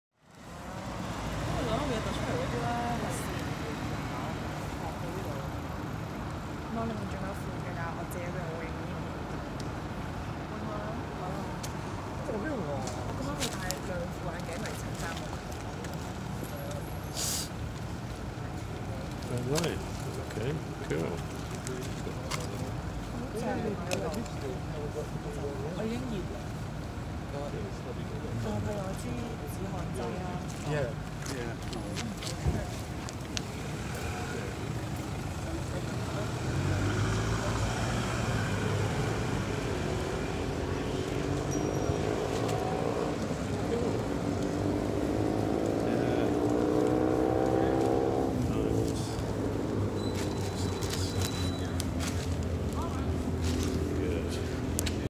New pedestrian crossing on The Mall
It is not working!! No cyclist pays any attention to the lights!!